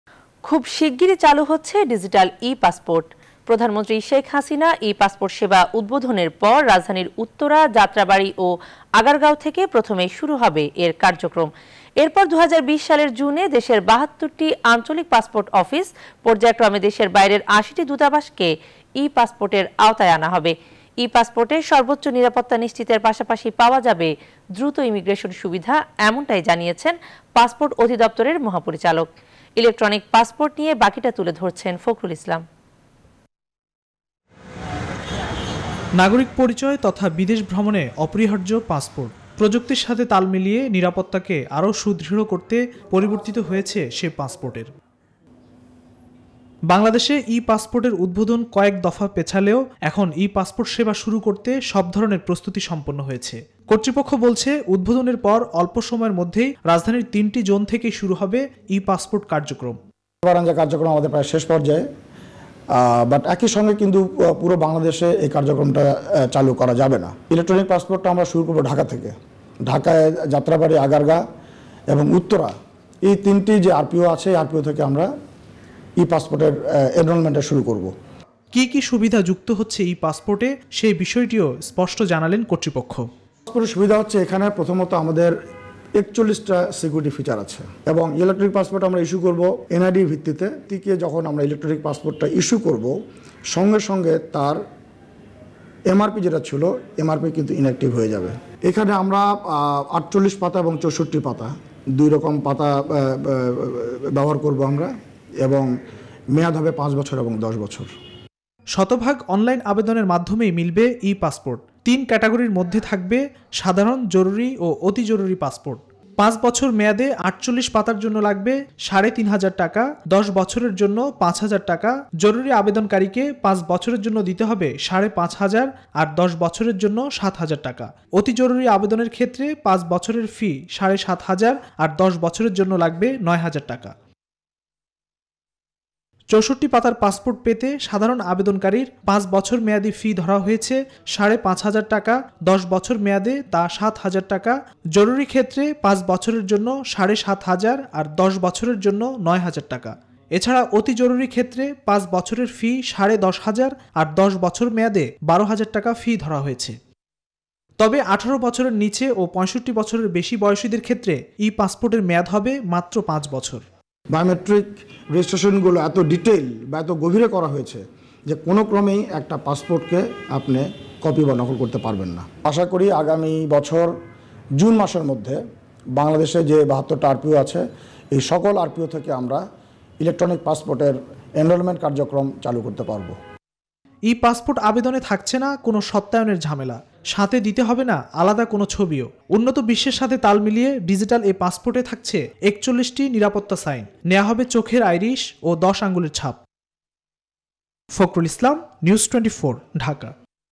খুব শিগগিরই চালু হচ্ছে ডিজিটাল ই-পাসপোর্ট 23Dec.19| e Passport Bangladesh
খুব শিগগিরই চালু হচ্ছে ডিজিটাল ই-পাসপোর্ট। প্রধানমন্ত্রী শেখ হাসিনা ই-পাসপোর্ট সেবা উদ্বোধনের পর  রাজধানীর উত্তরা, যাত্রাবাড়ী ও আগারগাঁও থেকে প্রথমে শুরু হবে এর কার্যক্রম। এরপর ২০২০ সালের জুনে দেশের ৭২ আঞ্চলিক পাসপোর্ট অফিস, পর্যাক্রমে  দেশের বাইরের ৮০ টি দুতাবাসকে ই-পাসপোর্টের আওতায় আনা হবে। ই-পাসপোর্টে সর্বোচ্চ নিরাপত্তা নিশ্চিতের পাশাপাশি পাওয়া যাবে দ্রুত ইমিগ্রেশন সুবিধা-এমনটাই জানিয়েছেন পাসপোর্ট অধিদপ্তরের মহাপরিচালক। ইলেক্ট্রনিক পাসপোর্ট নিয়ে বাকিটা তুলে ধরছেন ফখরুল ইসলাম।

Digital e-passport is being launched very soon. After the inauguration of e-passport services, Prime Minister Sheikh Hasina will start first from Uttara, Jatrabari and Agargaon in the capital. Then in June 2020 country's 72 regional passport offices and 80 embassy will be covered under e-passport. In addition to the highest security guaranteed in e-passport, rapid immigration will be available. Report - Fakhrul Islam.

On Aired on NEWS24 on 23rd December, 2019
Our other Youtube channels :
News24 Official Address:
NEWS24
371/A
Bashundhara Road
Block – D
Bashundhara Residential Area
Dhaka - 1229